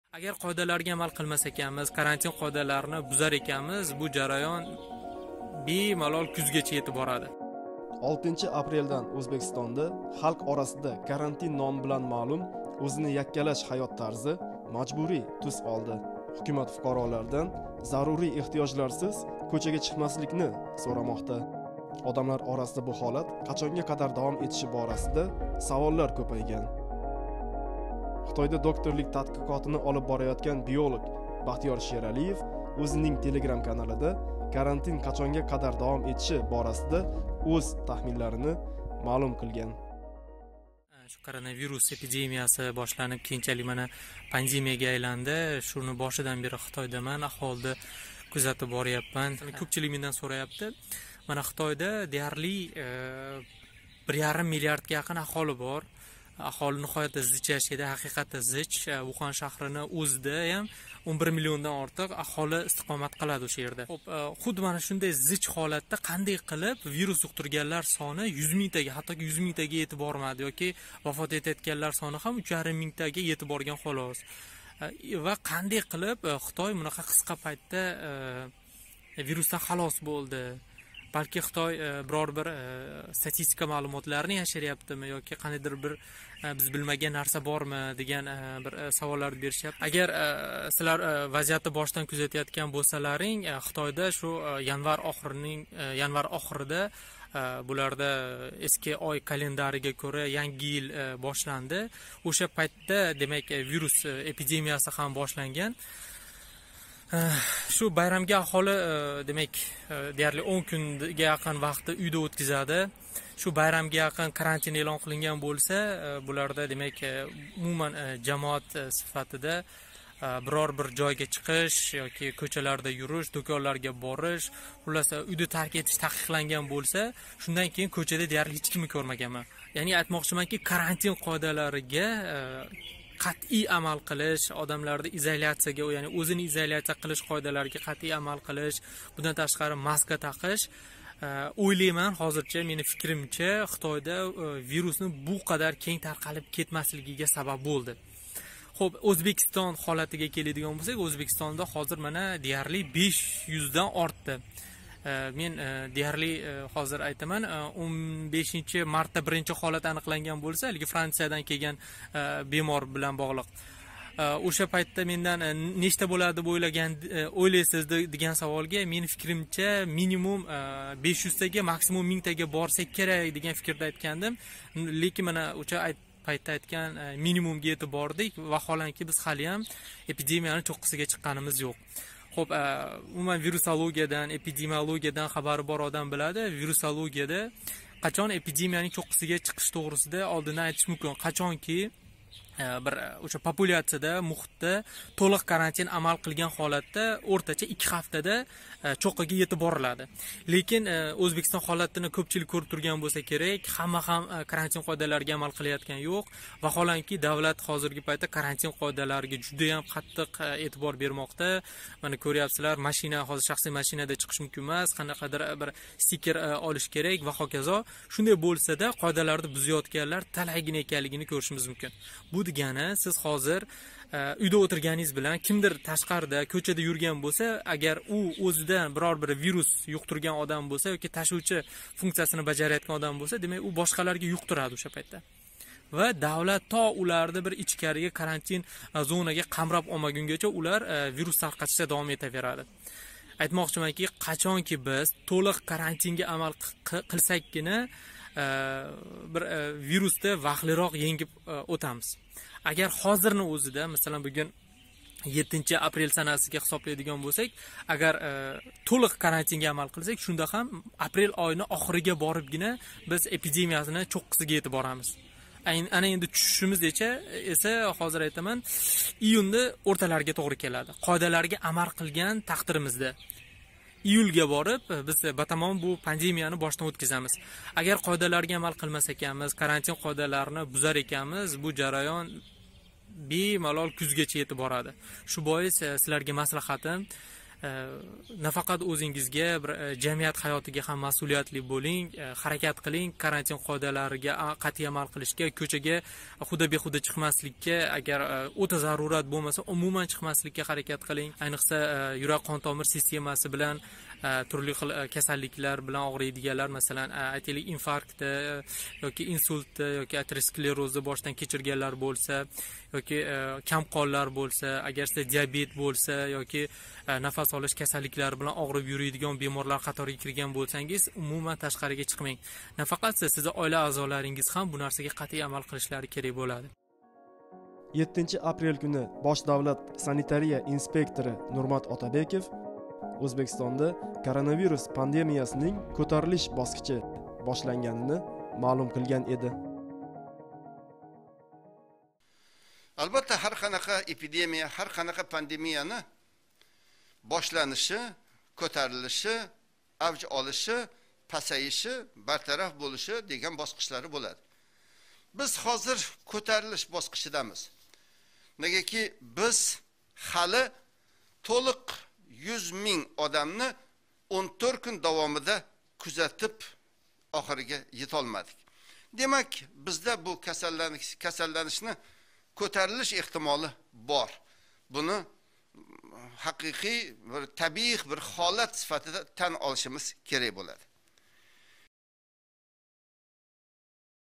Ўзбекистон, коронавирус: Эпидемия қачон ўз чўққисига чиқади? - BBC Uzbek
Ўзбекистонда 1 апрелдан жорий этилган «ўзини яккалаш» тартиби 6 апрелдан мажбурий тус олди. Расмийлар халқ орасида «карантин» номи билан маълум ушбу тартиб қачонгача давом этиши номаълум эканини билдирган бир пайтда, Хитойда тадқиқот олиб бораётган биолог Бахтиёр Шералиев бу борада ўз тахминларини билдирди